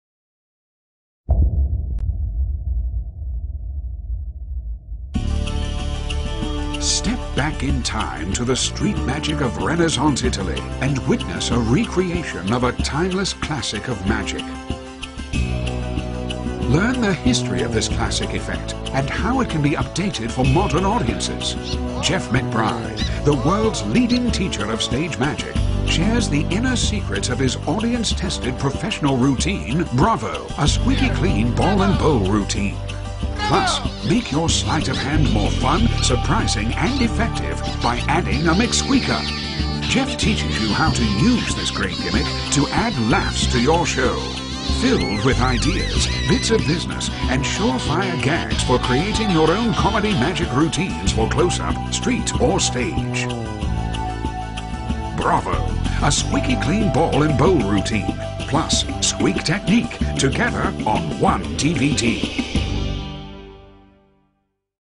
Squeak Technique Magic Trick DVD and Squeakers by Jeff McBride
Feel the Power of The Squeak!

Make your sleight-of-hand more effective by adding a "Mc-Sqeaker." Jeff teaches you how to use this great gimmick to add laughs to your show.

Filled with ideas, bits of business and sure-fire gags for creating your own comedy magic routines.

Includes BRAVO! A Squeaky Clean Ball and Bowl Routine.

The world's leading teacher of stage magic shares the inner secrets of a street magic classic! The inner workings now revealed!

Step back in time to the street magic of Renaissance Italy and witness a recreation of a timeless classic of magic. Learn now this time-tested routine can be adapted for today's audiences.

    This is a "do any time, any where" effect that will leave your audiences speechless!
    Includes the secret gimmicks that will enhance the routines and can be used in many other effects.
    Use your own or a borrowed bowl and sponges, or even rolled-up bank notes.
    Learn the history of this classic effect and how it can be updated for modern audiences.

Package includes two Mc-Squeakers.

Running Time Approximately: 1hr 43min

"If you want to enhance your close-up and stage routines with sound - something that I think just about any magician can benefit from - I recommend "Squeak Technique." And McBride's excellent "Benson Bowl" style routine is simply icing on the cake (actually, it alone is worth the price of the DVD)."
Wayne Kawamoto, Magic & Illusion 7th March 2012